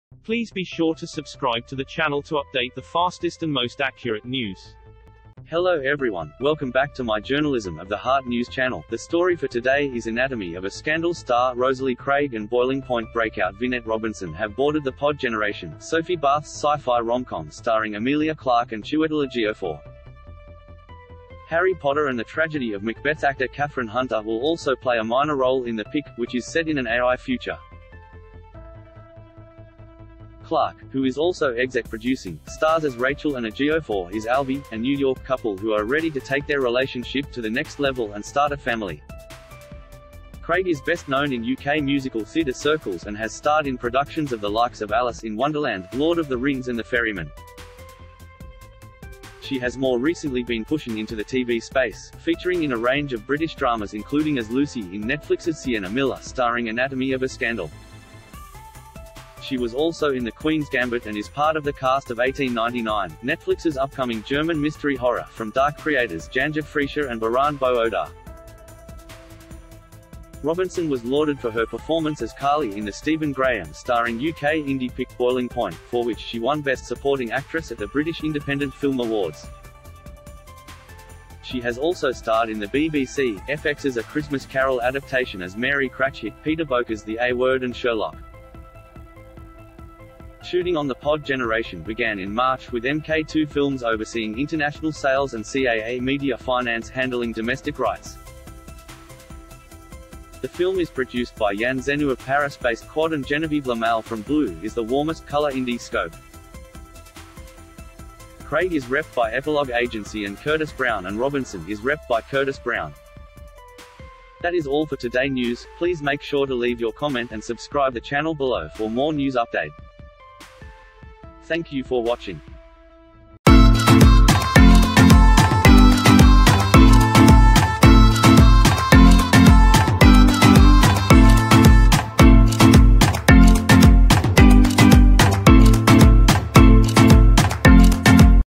‘The Pod Generation’ ‘Anatomy Of A Scandal’ Star Rosalie Craig And ‘Boiling Point’ Breakout
‘The Pod Generation’: ‘Anatomy Of A Scandal’ Star Rosalie Craig And ‘Boiling Point’ Breakout Vinette Robinson Board Sci-Fi Romcom Starring Chiwetel Ejiofor & Emilia Clarke